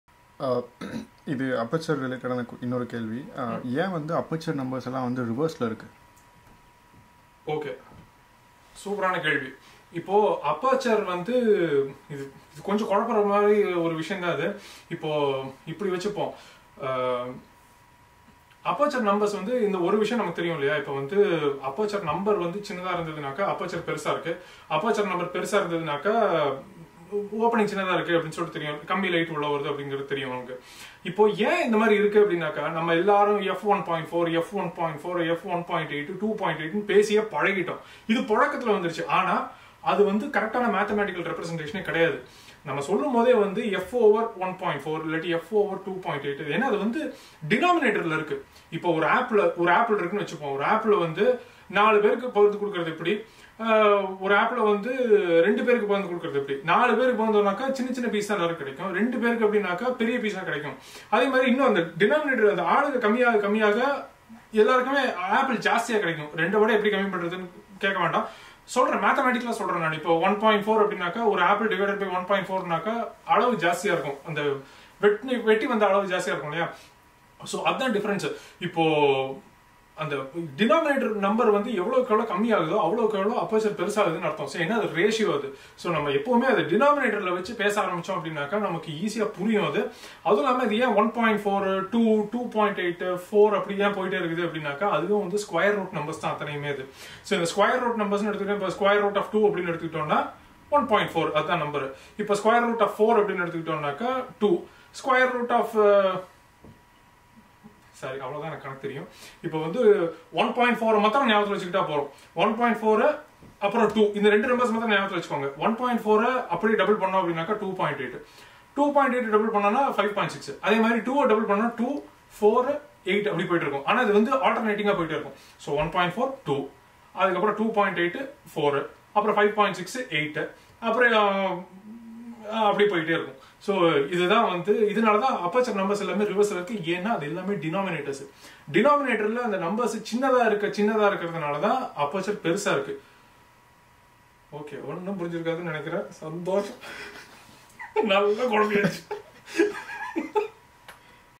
QA by Karthi 04 - Why Aperture numbers are reversed? | தமிழ் | Learn photography in Tamil
~~ Follow Me ~~

Thanks
Vinod